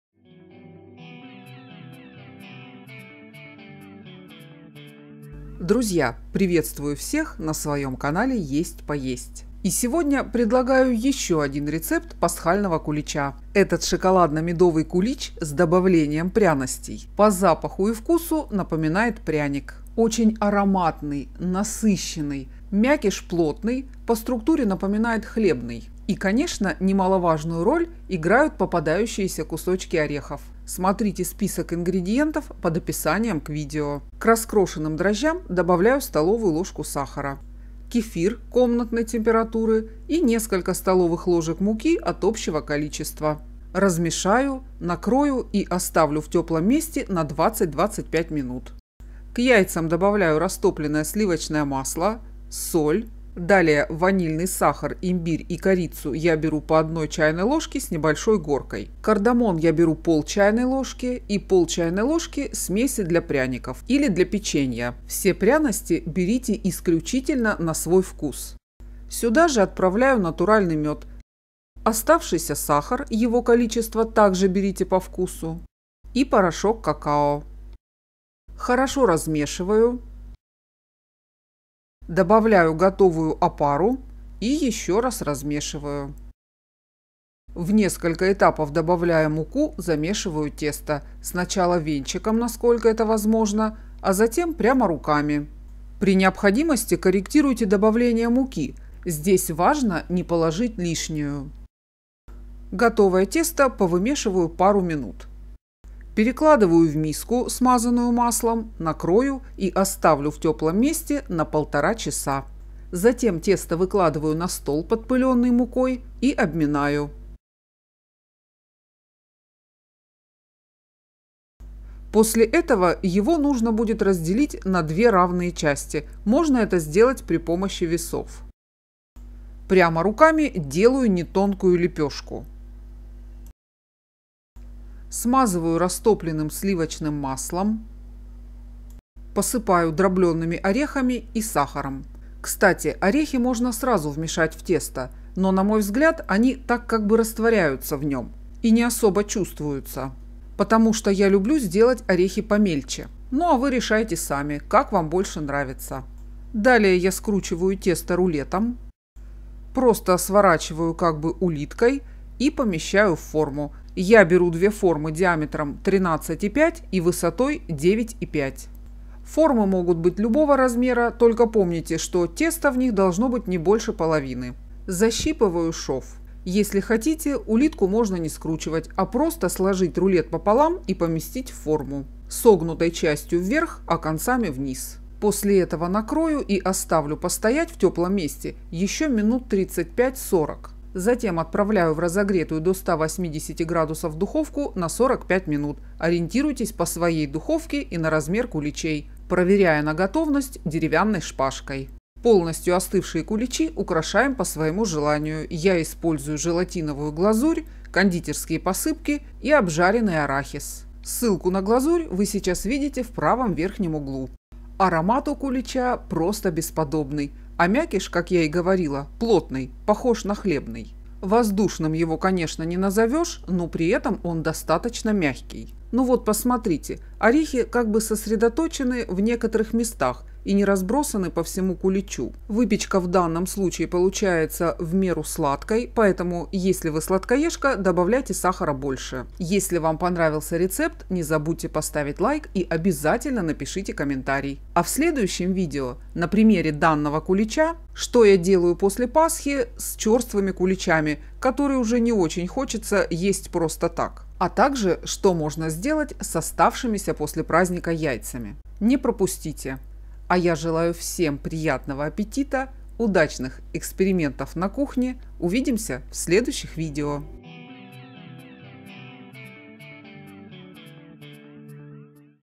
Кулич пряный шоколадно-ореховый.
Этот шоколадно-ореховый кулич с добавлением пряностей и мёда по вкусу и аромату напоминает пряники. Мякиш имеет плотную и при этом мягкую структуру. Все пряности кладите по вкусу. Я делаю тесто на кефире, но можно и на молоке.
ИНГРЕДИЕНТЫ
кефир 250 мл,
дрожжи сырые 20 г,
сахар 5 ст.л (или по вкусу),
мёд натуральный 60 г,
соль 1/2 ч.л,
порошок какао 20 г,
мука 500-550 г (ориентировочно),
ванильный сахар 1 ч.л,
имбирь сухой молотый 1 ч.л,
корица 1 ч.л,
кардамон 1/2 ч.л,
смесь пряностей для пряников 1/2 -1 ч.л,
коньяк (ликёр) по желанию.
Начинка:
масло сливочное 20 г,
орехи молотые (обжаренные) 80-100 г,